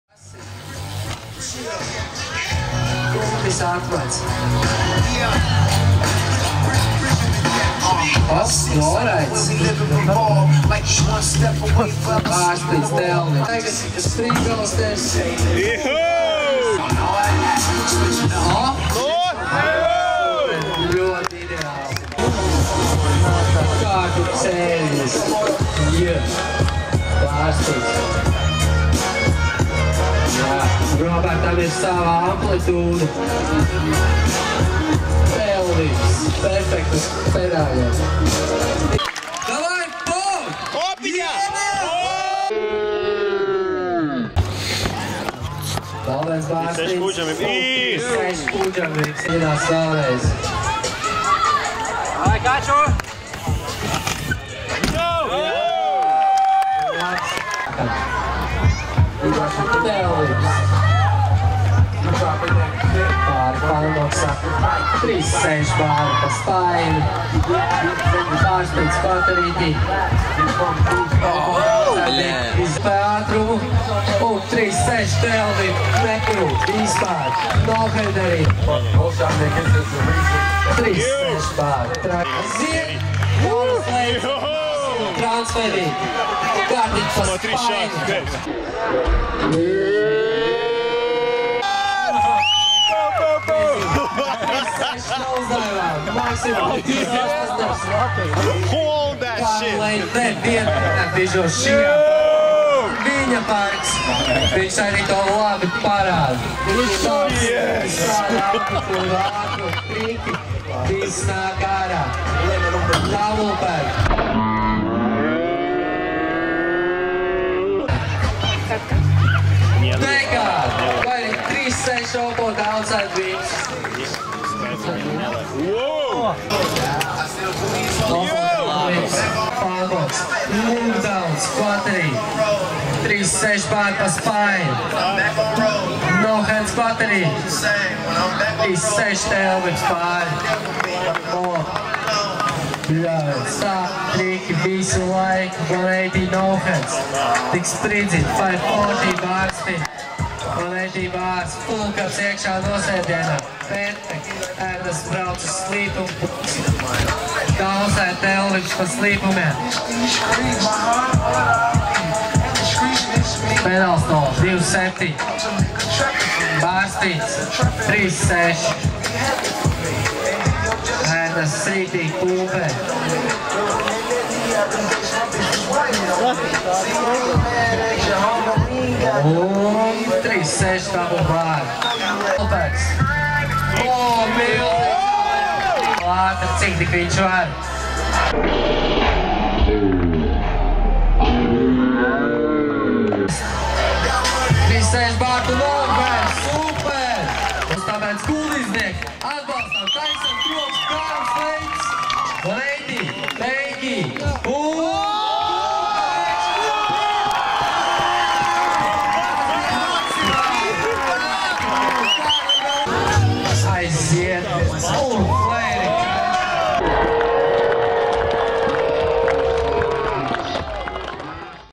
PARBMX / Kuldīga Open 2016
A few clips from the pro contest at the annual Kuldīga Open event. Good times.